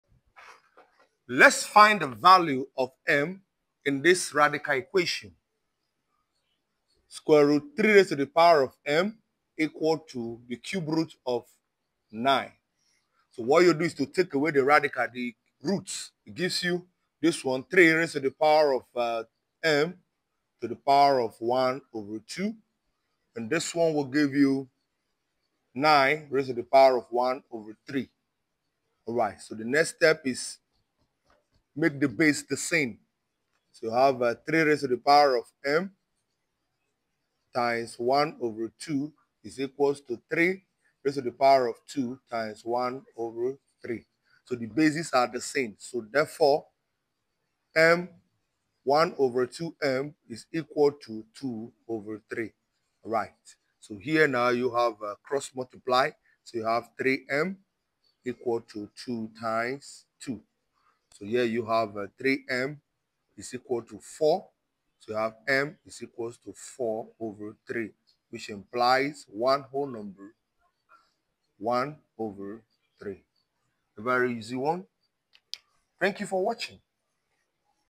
Radical Exponential Equation
Learn how to tackle radical exponential equations with ease in this advanced math tutorial. Discover the math tricks and tips you need to solve these complex equations, and master algebra help concepts like exponential growth. Whether you're looking for math tutorials to help you with equation solving or simply want to improve your math skills, this video is for you. We'll break down the steps to solving radical equations, so you can confidently approach even the toughest problems. Get ready to take your math skills to the next level!